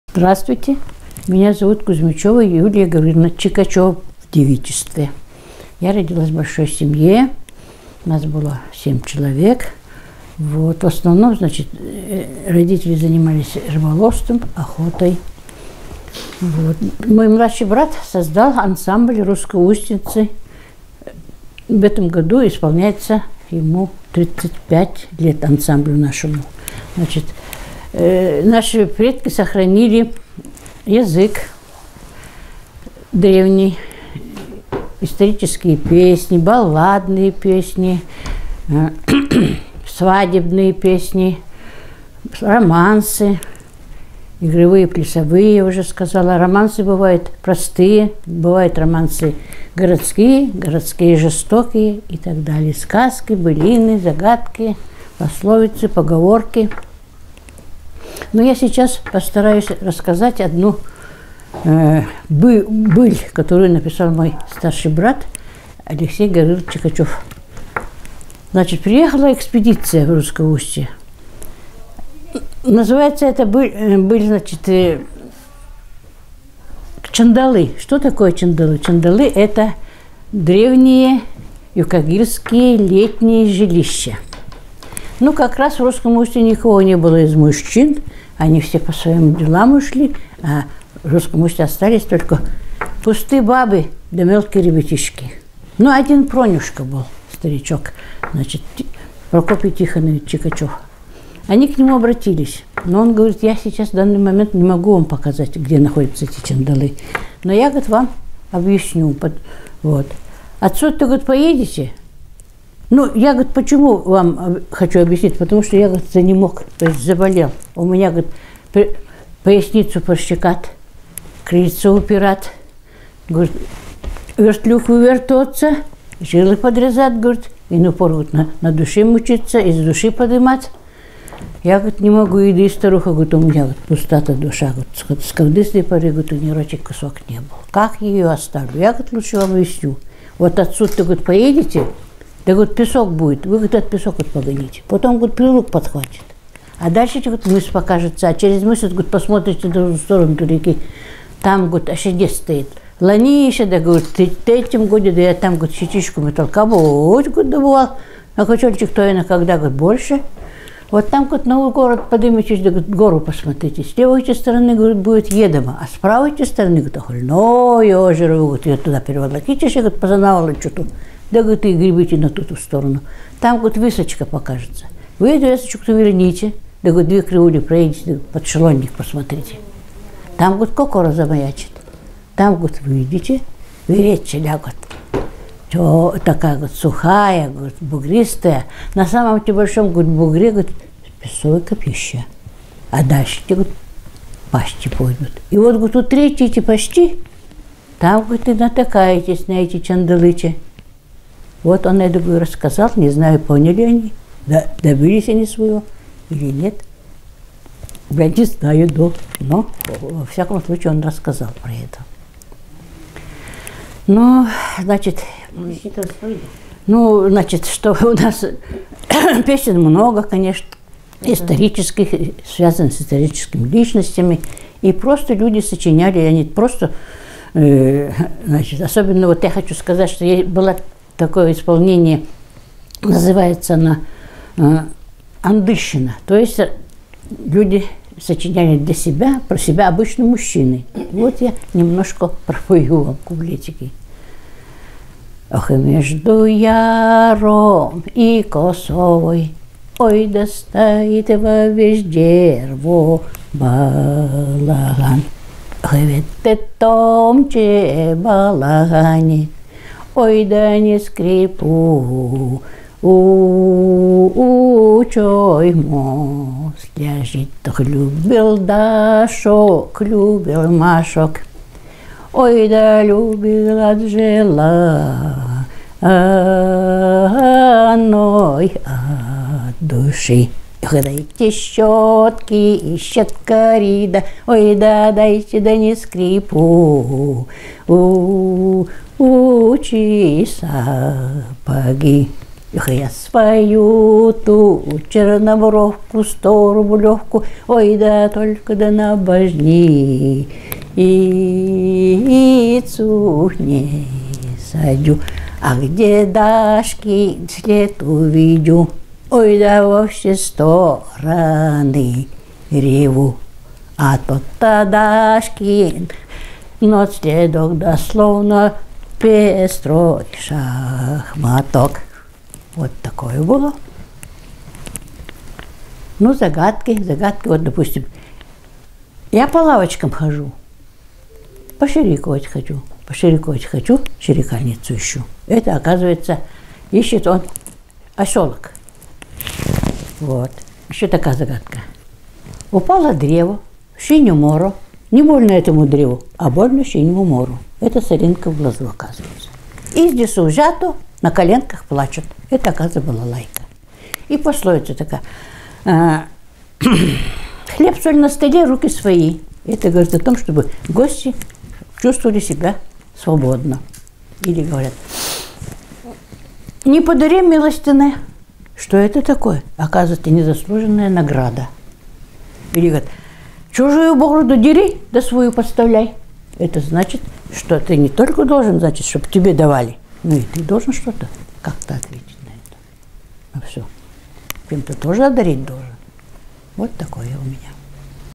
Кузмичева Юлия Гавриловна
Ансамбль "Русское Устье", п. Чокурдах, Аллаиховский улус, Республика Саха (Якутия). (дкабрь 2022 г.)